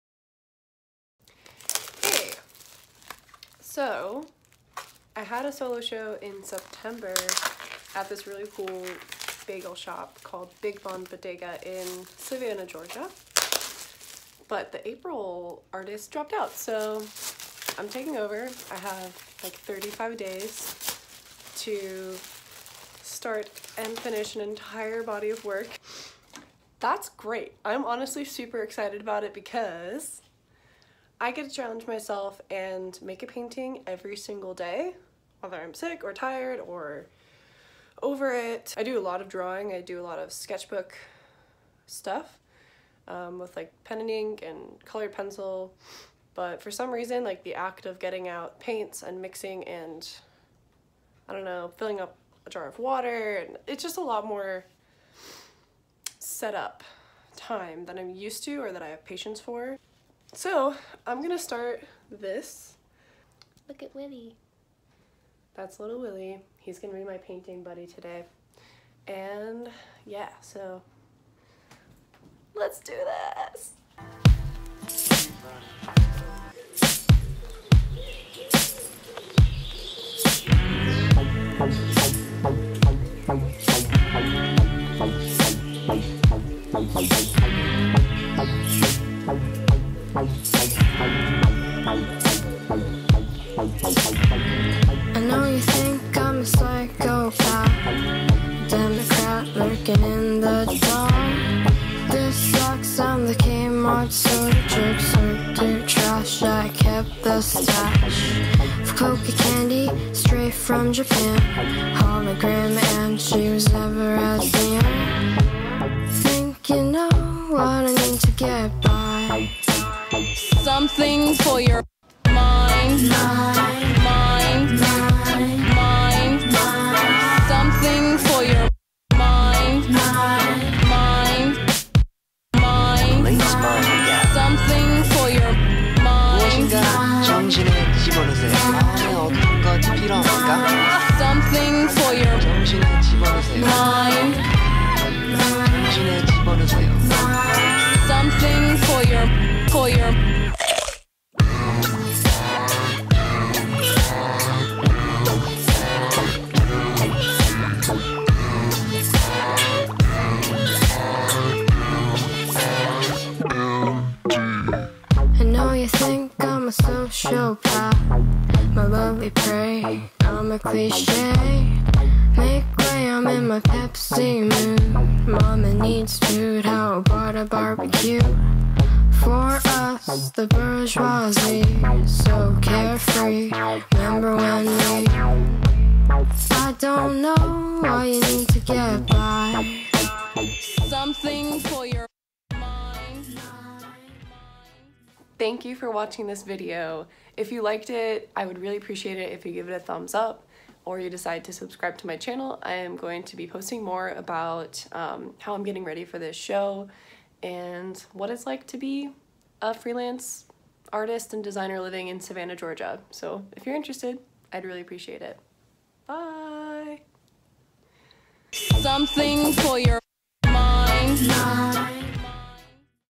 Art Vlog 001 - Art Show Prep
Hello! My name's Liv and I'm a freelance illustrator and designer living in Savannah, GA. I'm getting ready for my first solo art show coming up and wanted to take you along for the ride!

In this video, I do a bit of brainstorming, have a few aha moments, figure out the title and concept behind my show, then set out to make my first piece. I've time-lapsed my process so you can see how I work in LIGHT speed.

MATERIALS:
- Wood blocks from The Home Depot
- Acrylic Paint
- BEHR Paint Samples
- Random Brushes
- BLICK Artist Tape